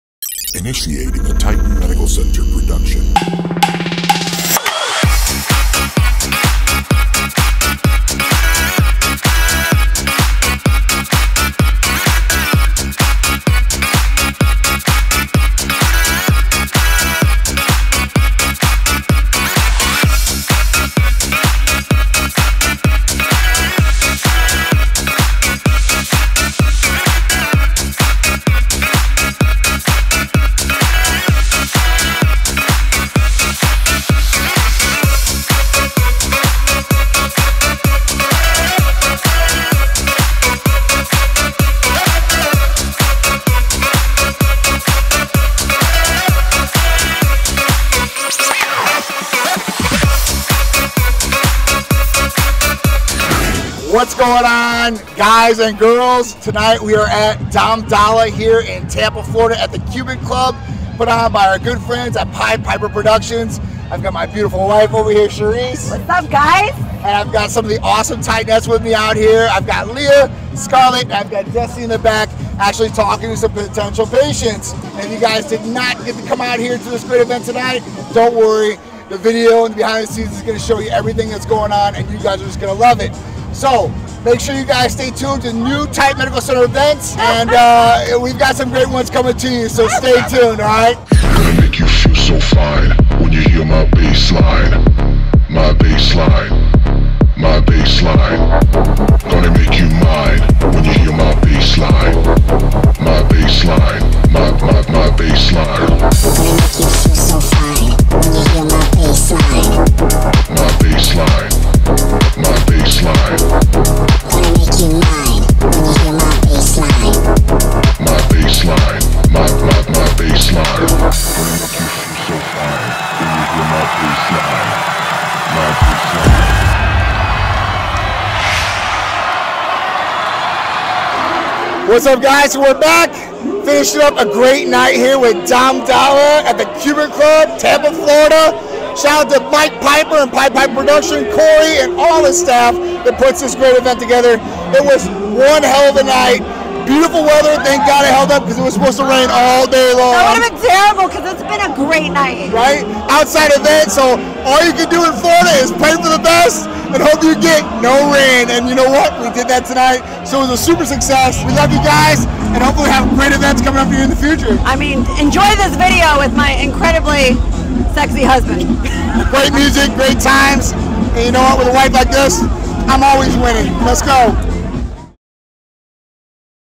Dom Dolla at the Cuban Club in Tampa by Pied Piper productions!
Titan was proud to sponsor the @PiedPiperProductions @DomDolla concert at the historic @CubanClubTampa in Tampa!